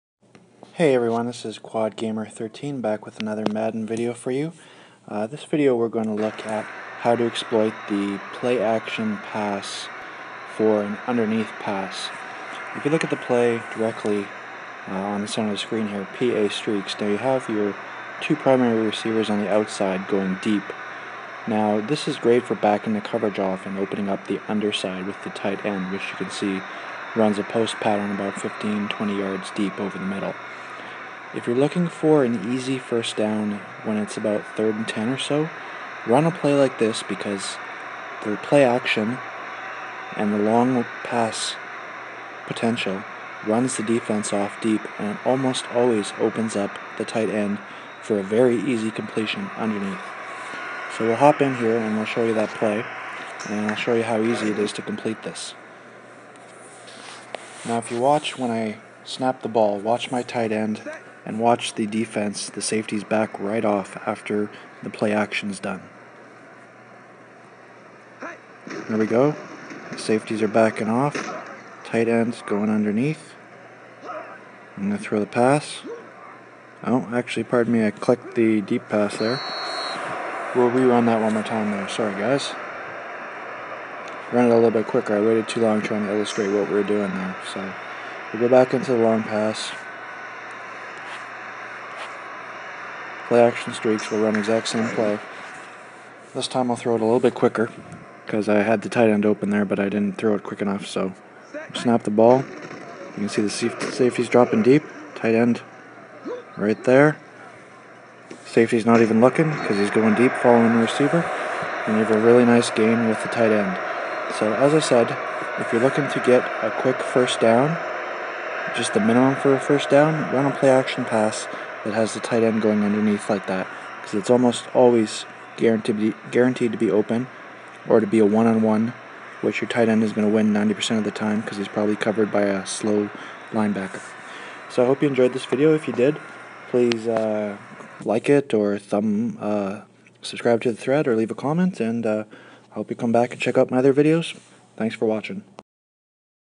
Working the tight end underneath off of a deep play action
This is a how-to tutorial on how to throw to the tight end underneath coverage off of the deep play action pass on the PA streaks pass play